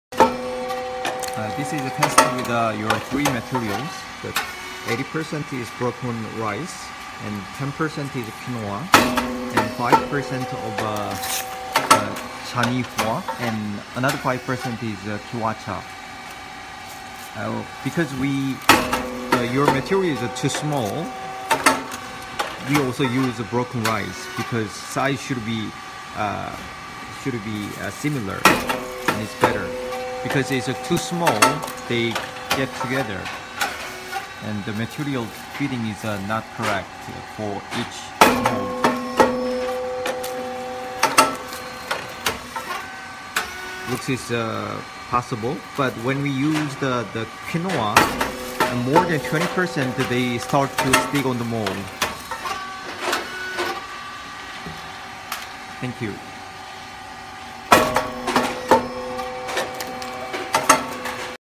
Test of SYP4506, Canihua, Kiwicha and Quinoa mixed with Rice
Three grains, Canihua, Kiwicha and Quinoa, were mixed with rice and tested. The model used is SYP4506 Rice cake machine, and three grains were mixed with rice for 5 ~ 25%.

This test is a mixture of three grains and rice.
For reference, rice was crushed to make it similar in size.

Canihua 5%
Kiwicha 5%
Quinoa 10%
Broken Rice 80%